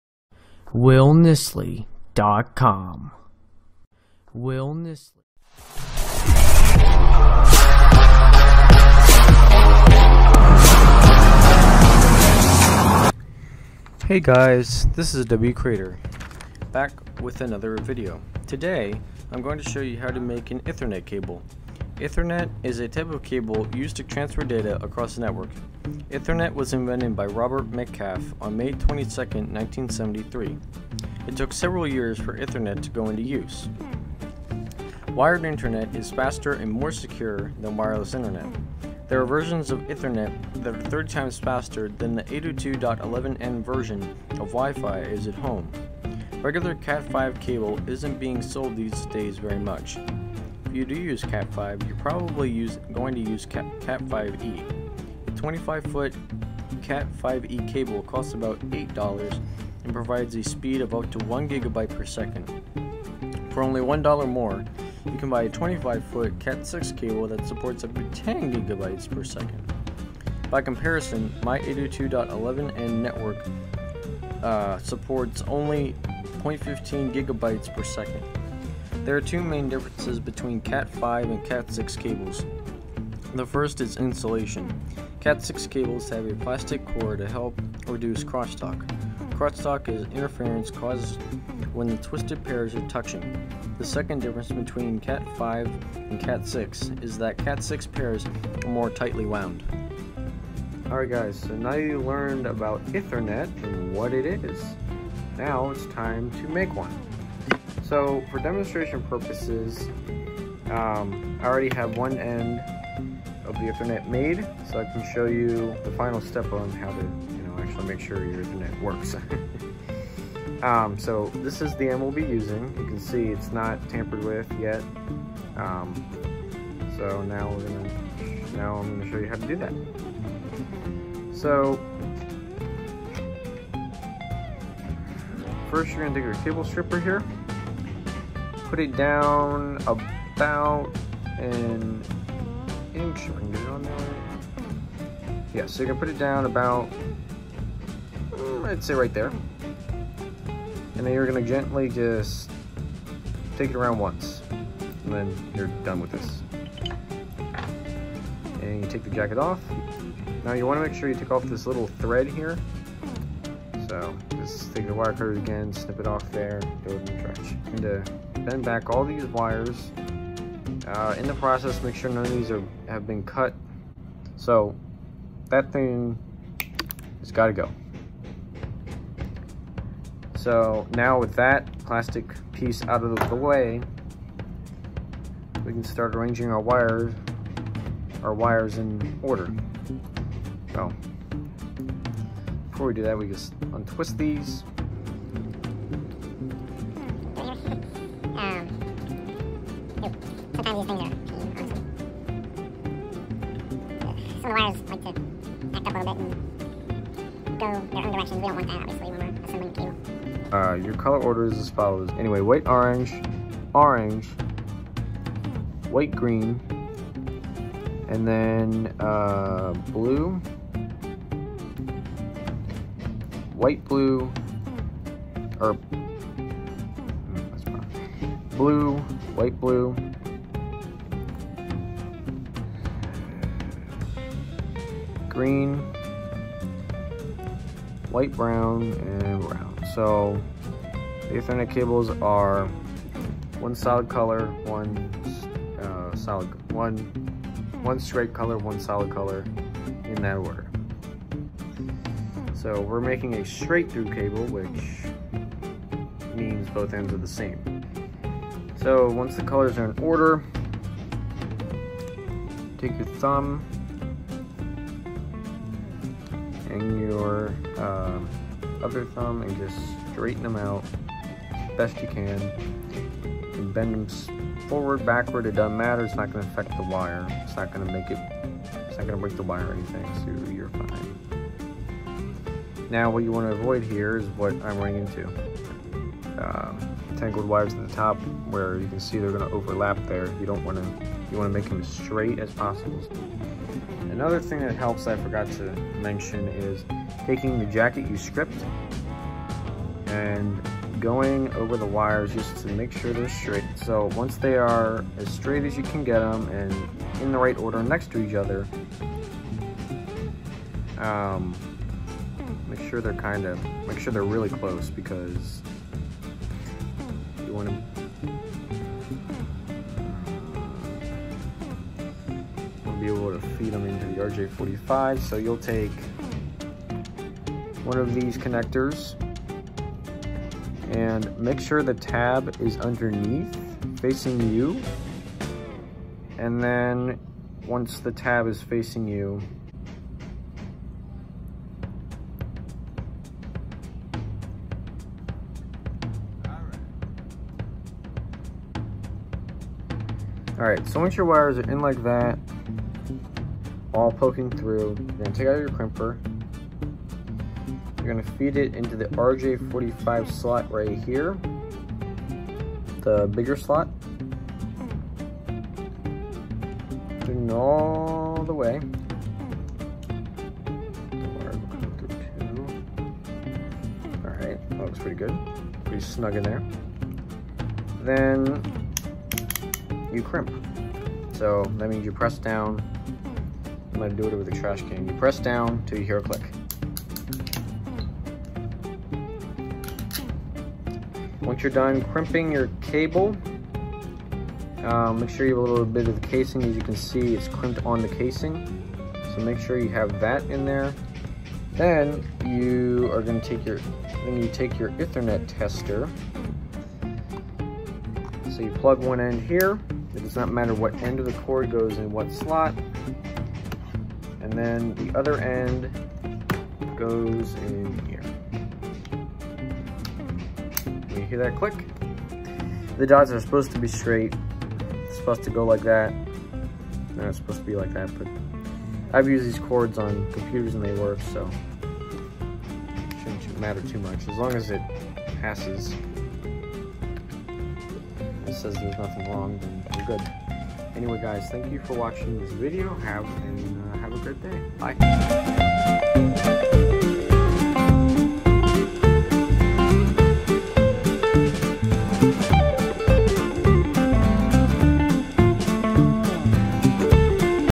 How to Make an Ethernet Cable
In this video, you learn what an ethernet cable is and how to make one.
I do not own ant of the music in this video. All rights are reserved to the creators.